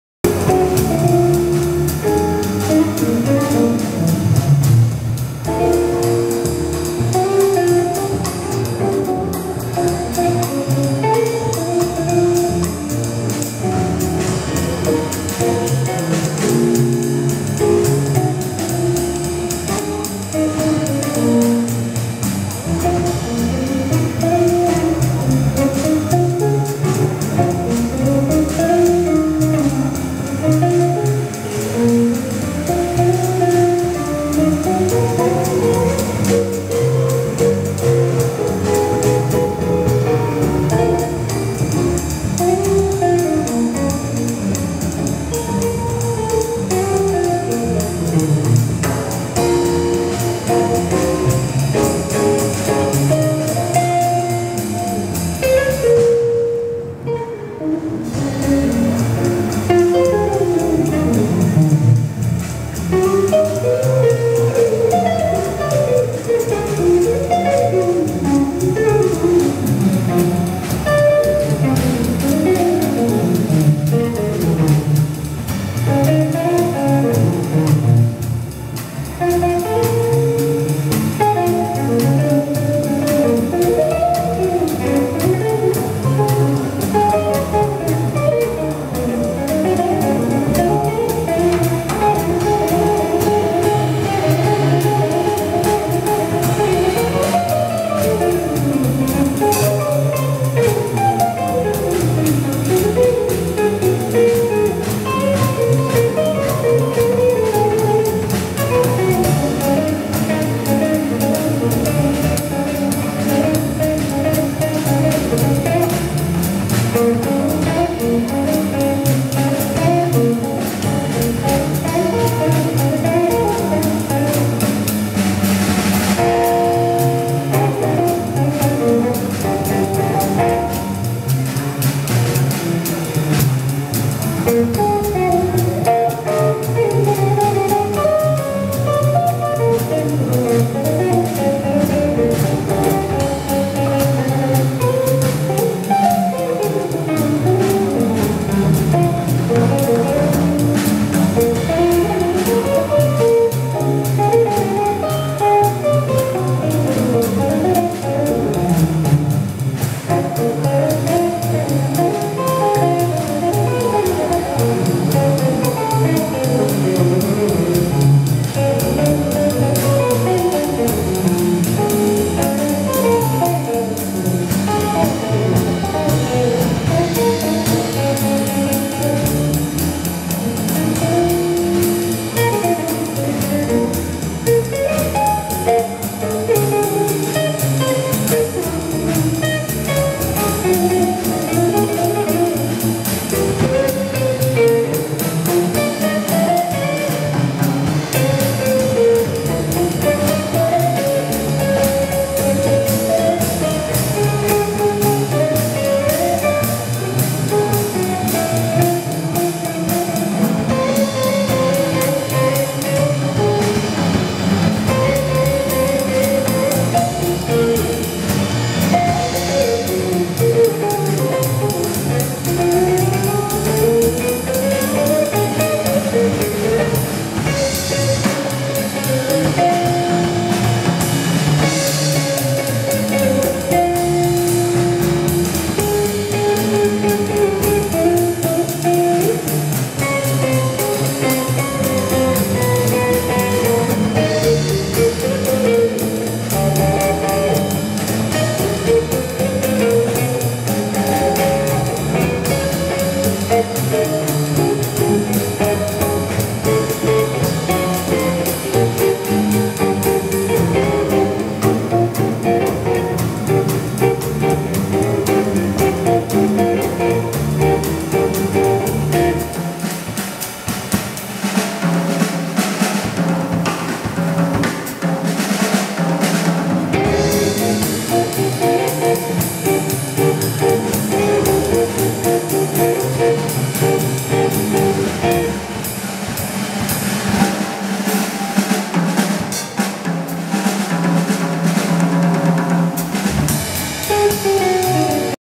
Joseval Paes & Amigos tocando na série Música no Aeroporto 2015
Joseval Paes & Amigos tocando na série Música no Aeroporto 2015, produção musical e curadoria de Sérgio Freddi e Arbra Arte e Música